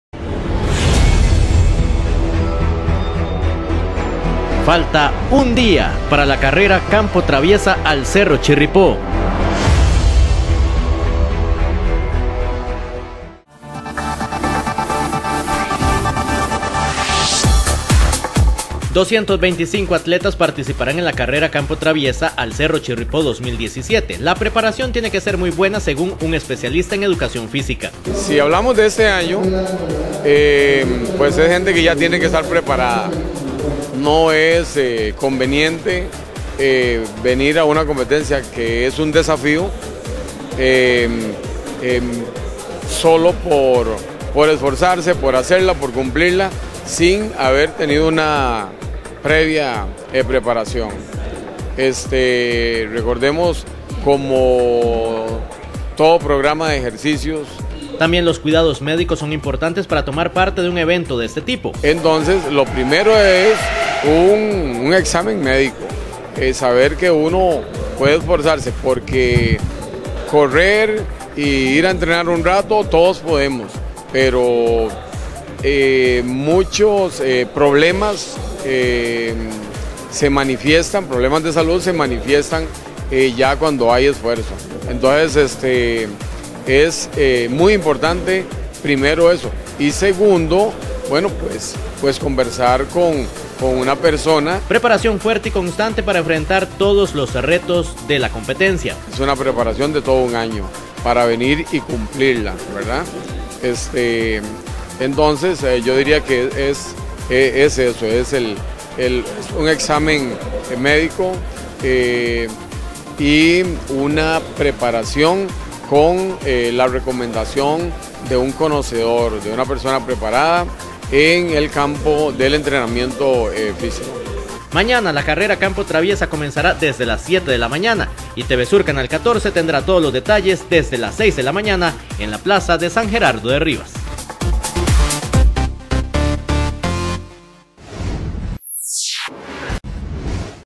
Atletas se preparan por lo menos seis meses para enfrentar el Chirripó
. 225 atletas participaran en la Carrera Campo Traviesa el Chirripo 2017 este sábado 25 de febrero, la preparación tiene que ser buena según Minor Chinchilla quien es especialista en educación física